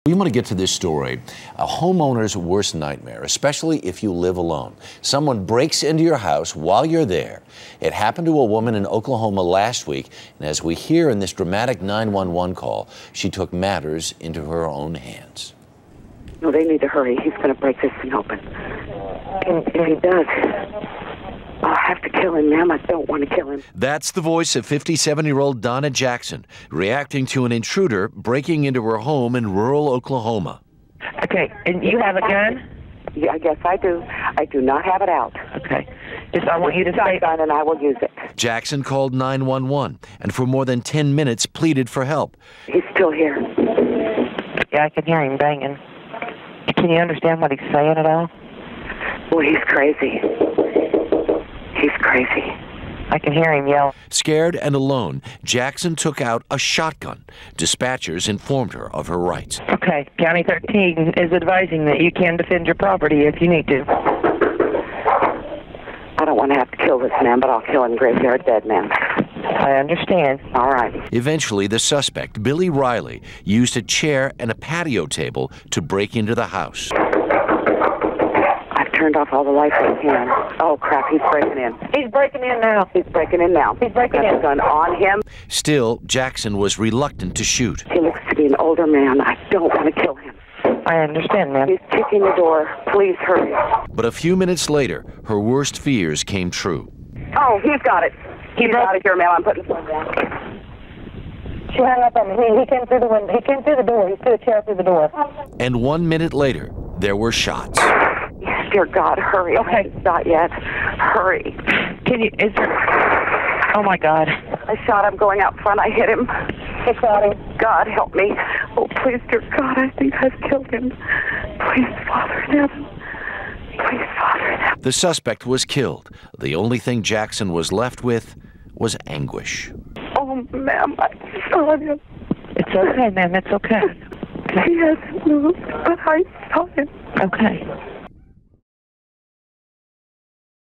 Woman on 911 Call Shoots Intruder
An emergency 911 call recorded the 10 minute ordeal of a rural Okla. woman who shot and killed an intruder to her home while waiting for authorities to arrive.